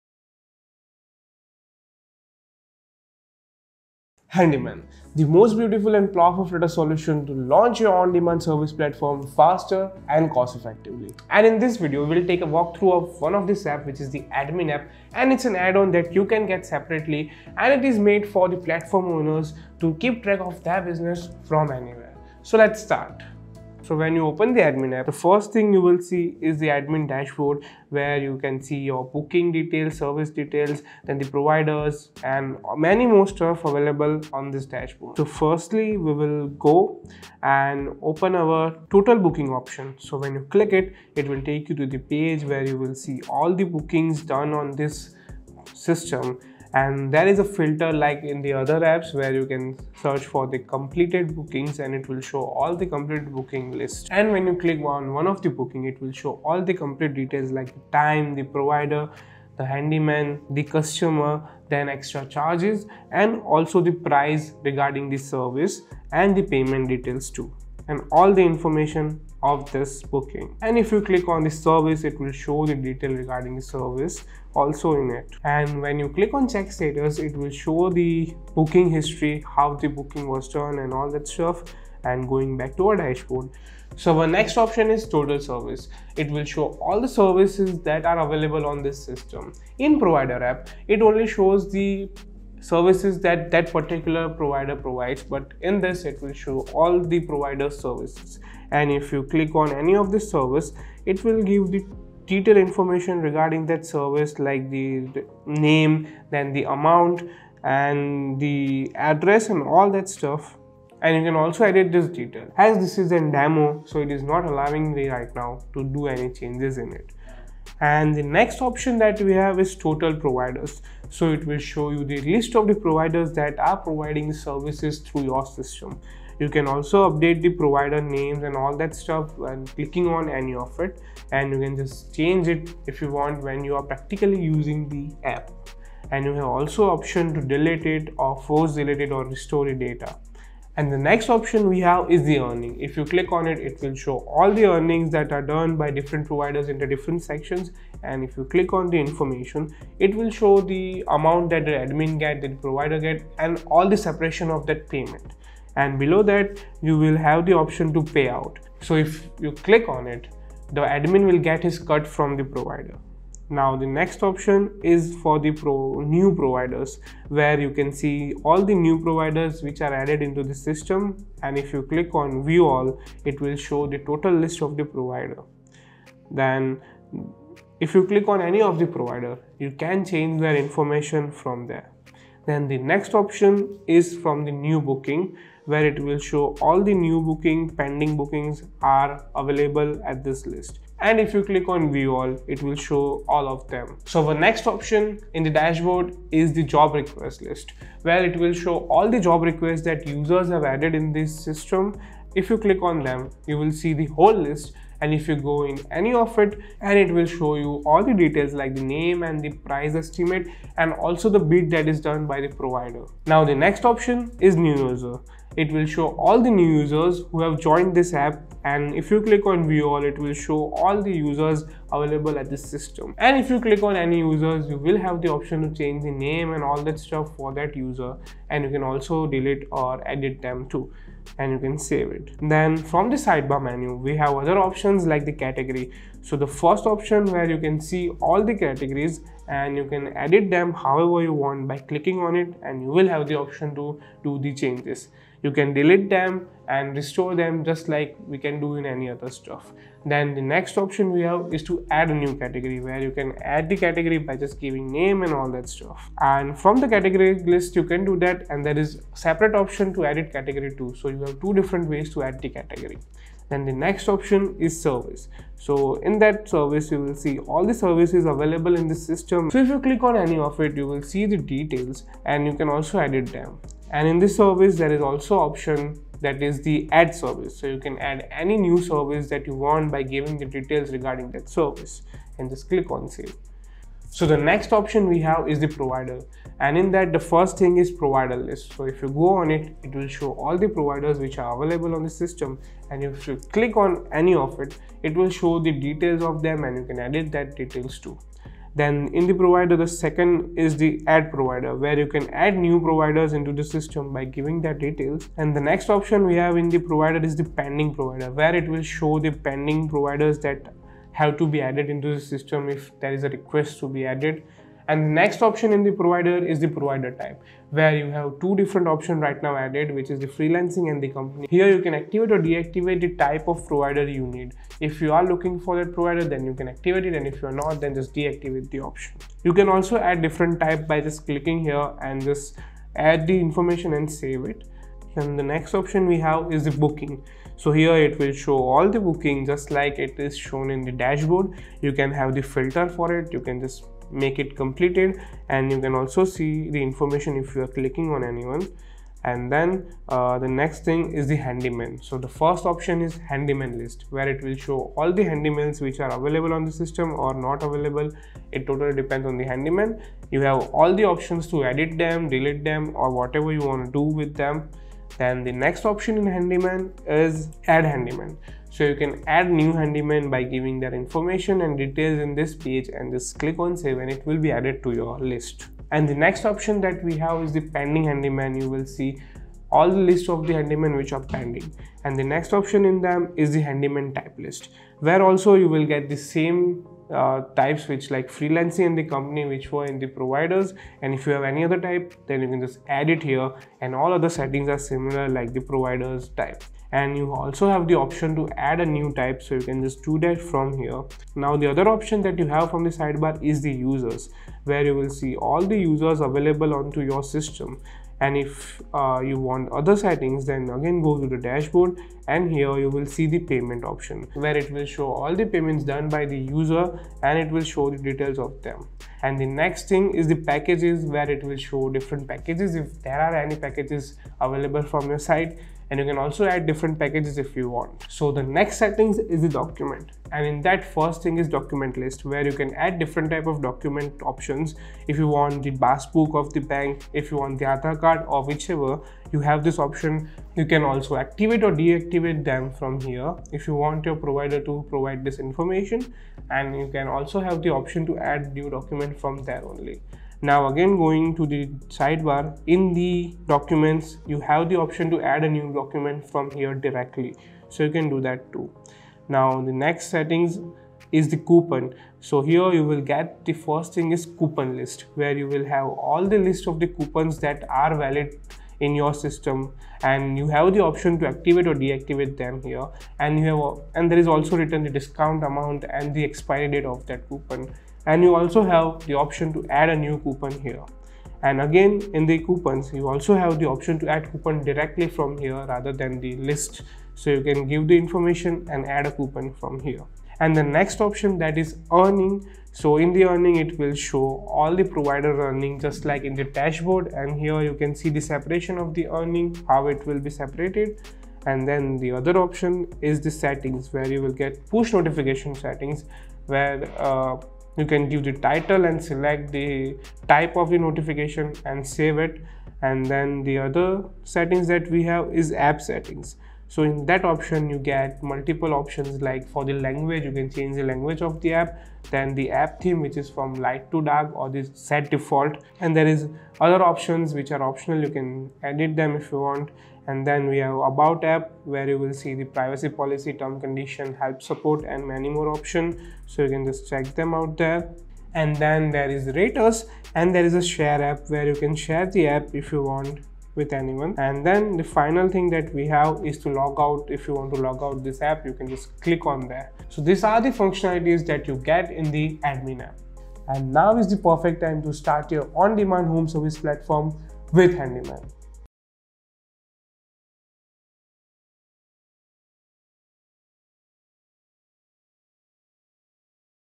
Learn About The Admin App And Dashboard Of Handyman | Iqonic Design
Welcome to this video about the Admin App and Dashboard of Handyman! In this video, we will give you an overview of the powerful tools and features available through the Handyman admin app and dashboard.

Our app and dashboard provide an easy-to-use interface for users to manage their business, from accepting and completing orders to managing their service offerings and pricing. You'll see how our intuitive interface streamlines the process of managing orders and services, making it easy for service providers to stay organized and efficient.

In addition to the basics, we'll also show you how the app includes tools to help service providers reach new customers, build their brand, and generate more revenue.

Whether you're currently a user of Handyman or just interested in learning more about our platform, this video is a must-watch. You'll come away with a deep understanding of how the Handyman admin app can help you take your business to the next level.

So sit back, grab your notebook, and let's dive into the world of the Handyman!